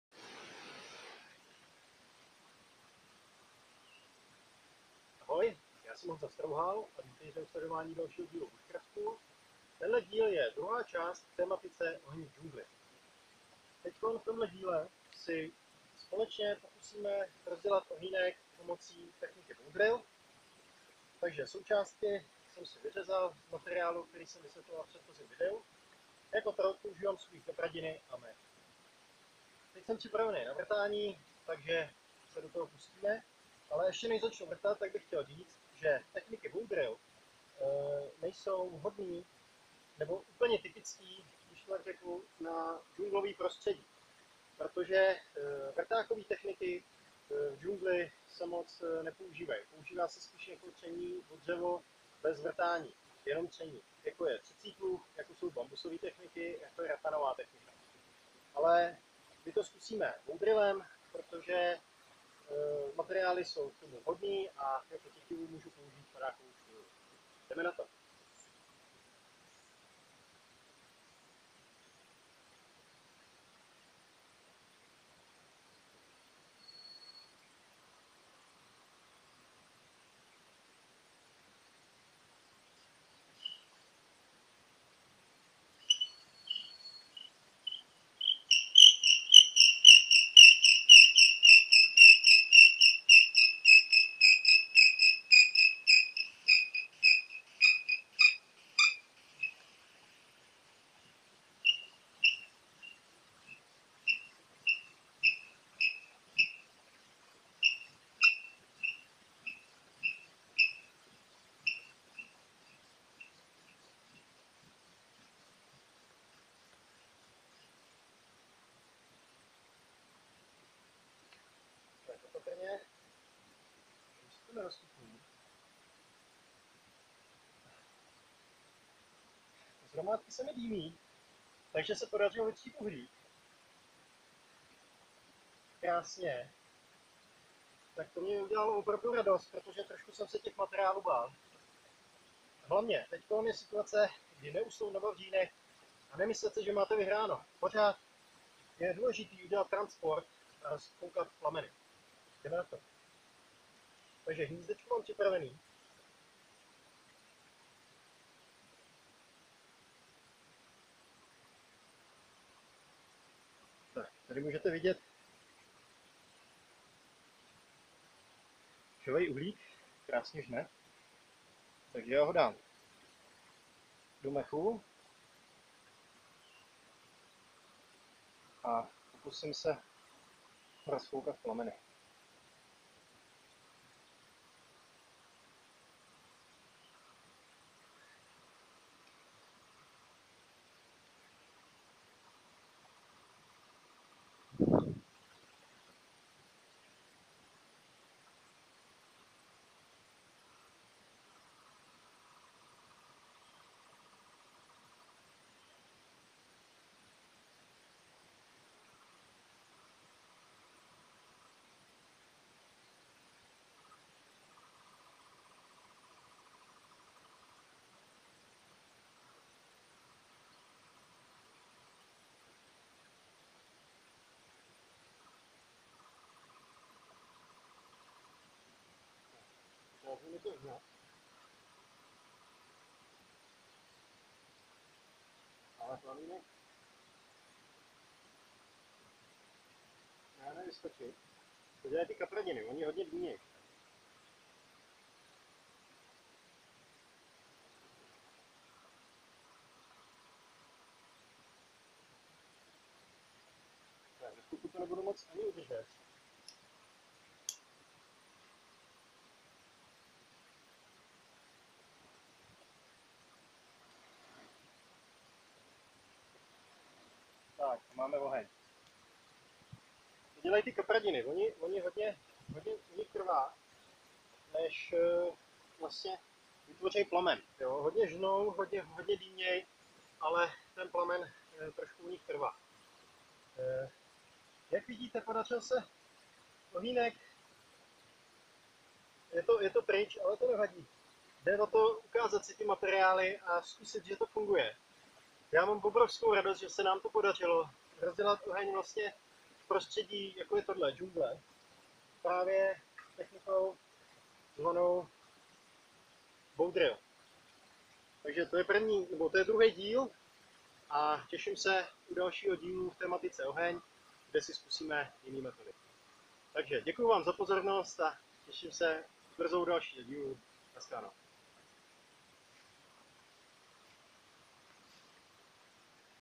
OHEŇ V JUNGLI DÍL II. BOW DRILL
Vydejte se se mnou do nového prostředí, kde společně objevíme tajemství ohně, ukryté v tropickém dřevě.
Vyvrtání žhavého uhlíku bylo mnohem složitější než v lesích mírného pásu a to zejména kvůli všudypřítomné vlhkosti. Ani troud nebyl "to pravé ořechové" obecně však kapradiny vytváří více dýmu než například  vršky suchých travin a  malinko déle u nich trvá vznik prvních plamínků. Podobně se chovají i chomáče chmíří bodlačí a orobince.